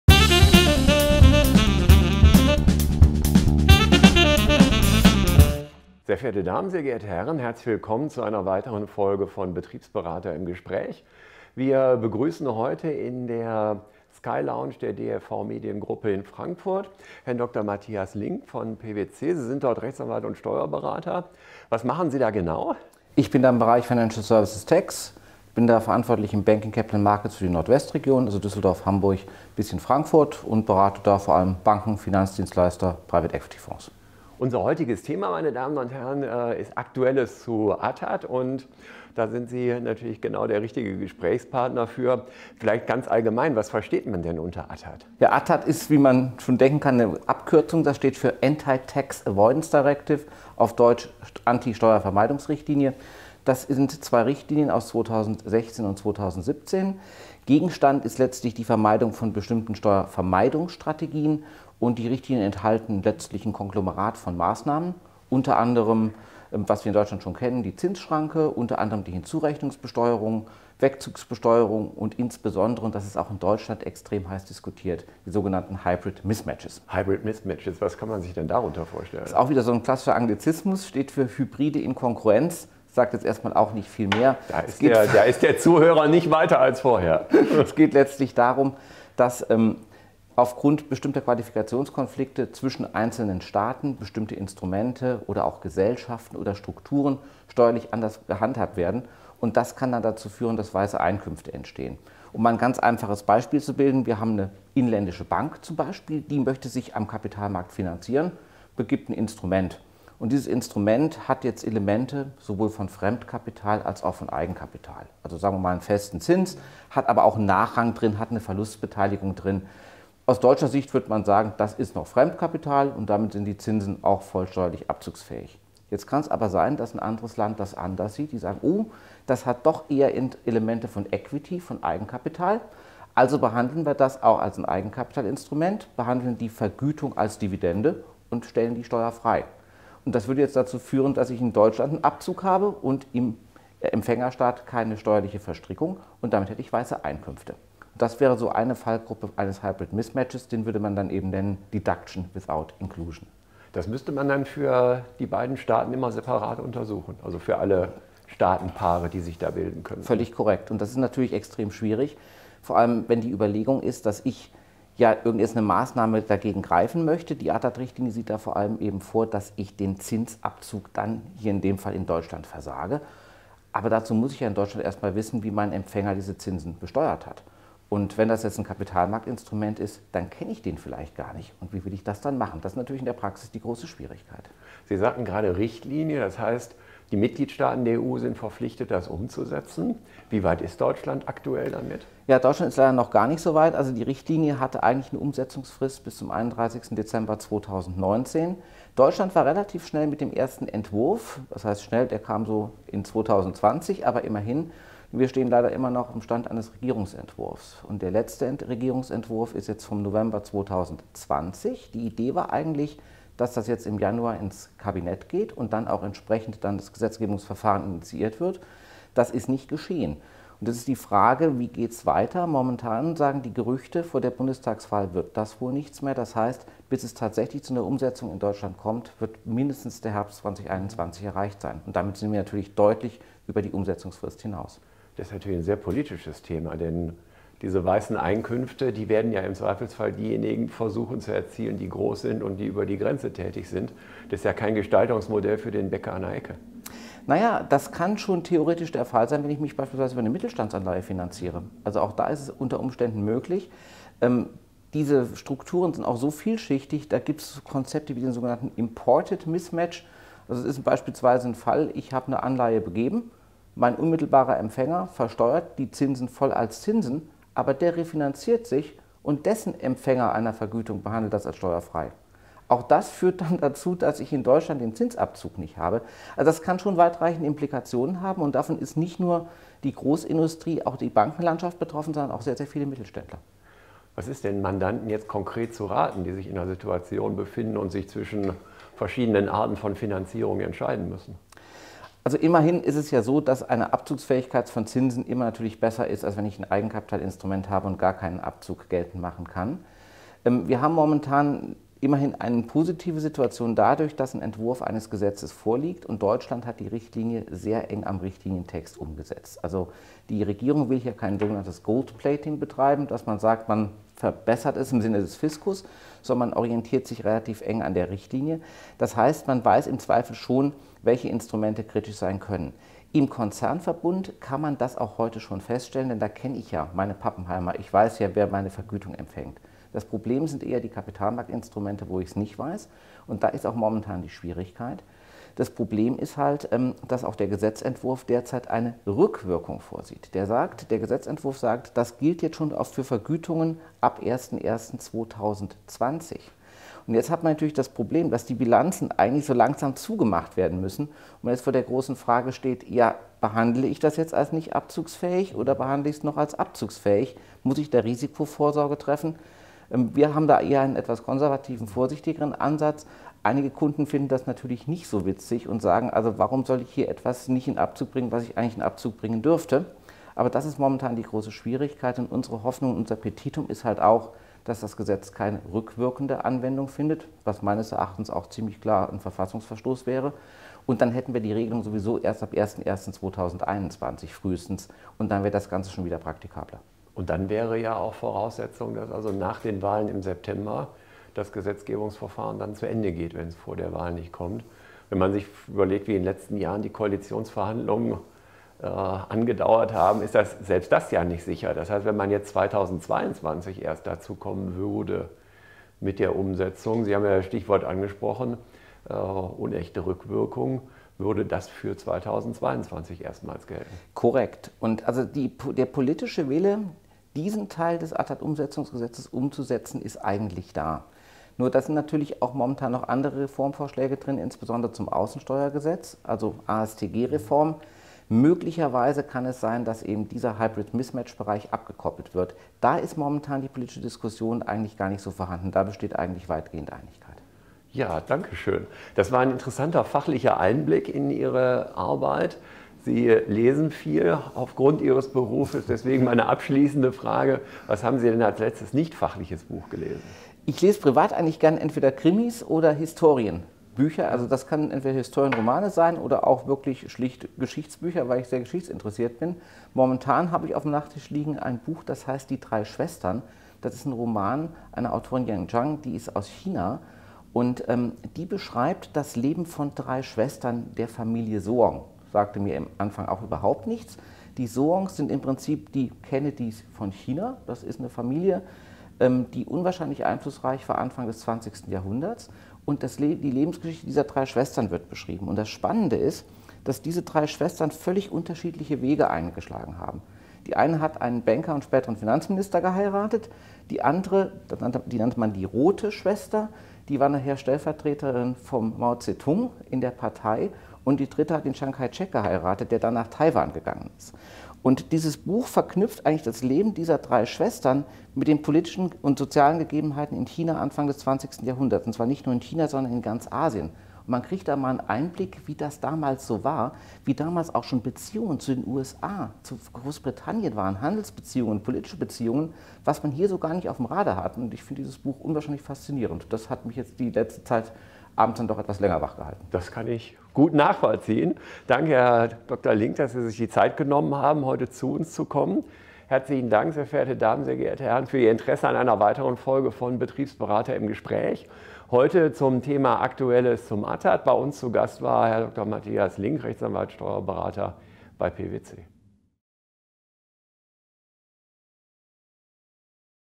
BB im Gespräch mit Dr. Mathias Link, Partner bei PwC
"Vor der Bundestagswahl wird das wohl nichts mehr." Prof. Jens M. Schmittmann im Gespräch mit Dr. Mathias Link über den Stand des ATAD-Umsetzungsgesetzes in Deutschland.